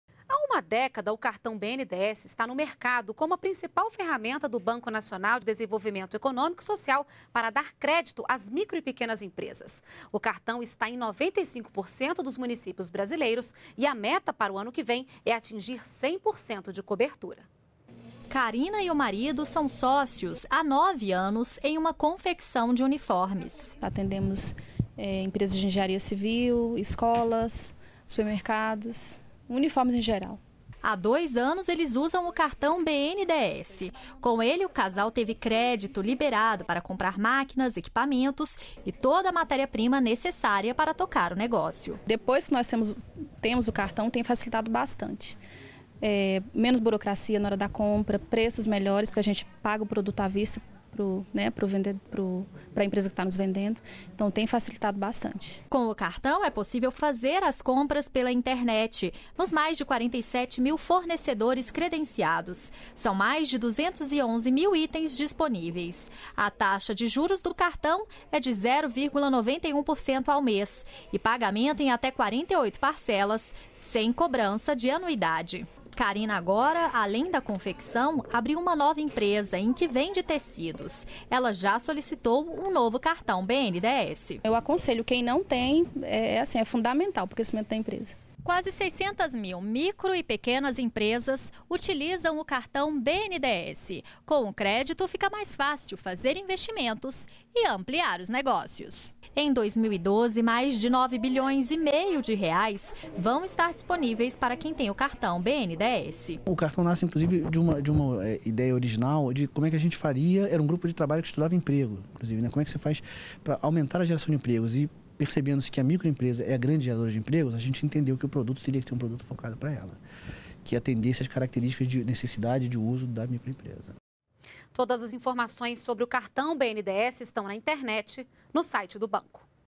Cerca de 600 mil micro e pequenas empresas já receberam crédito no cartão do BNDES, em 10 anos
NBR NOTÍCIAS - 04.12.12: O cartão do Banco Nacional do Desenvolvimento Econômico e Social (BNDES) está presente em 95% dos municípios brasileiros e é considerado a principal ferramenta do BNDES para financiamento e já concedeu crédito a cerca de 600 mil micro e pequenas empresas nos dez últimos anos. A meta em 2013 é atender a 100% dos municípios e gerar mais empregos para a população. Informações no